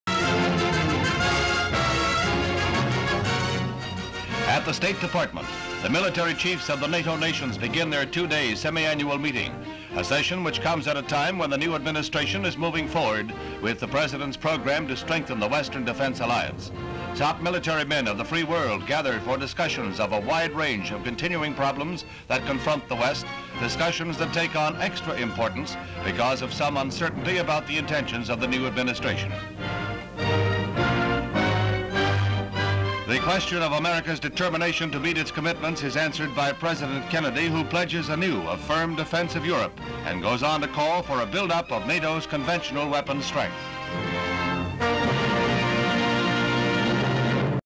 JFK calls for weapons build up at 1961 NATO Meeting newsreel archival stock footage
JFK calls for weapons build up at 1961 NATO Meeting